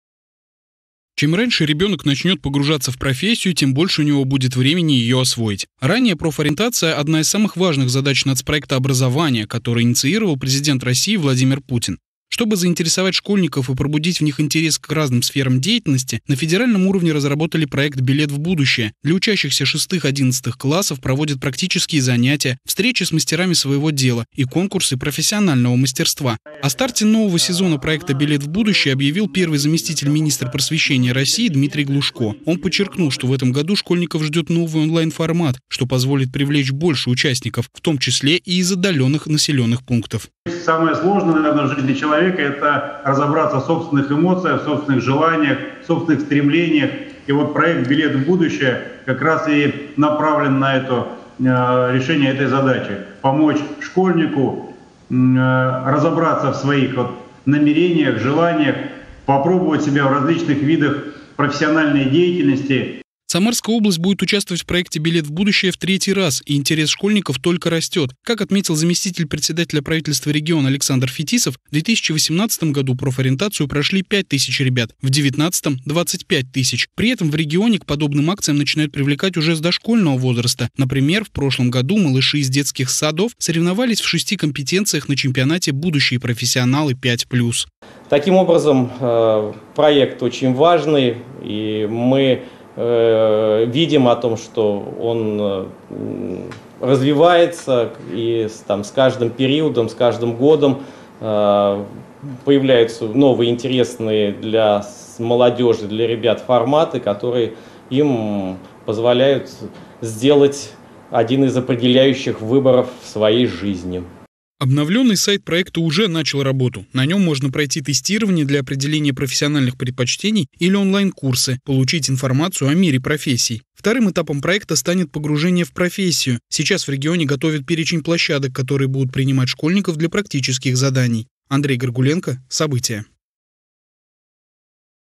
В регионе стартовал Всероссийский проект "Билет в будущее"
В этом году на региональный рынок труда выходят 27 тысяч выпускников вузов и колледжей региона. В среднем доля новоиспеченных специалистов, устроившихся по своему профилю - около 70 %. Повысить этот показатель может ранняя профориентация. В регионе стартовал всероссийский проект "Билет в будущее". Когда школьники смогут познакомиться поближе со всем многообразием  профессий, расскажет Андрей Горгуленко.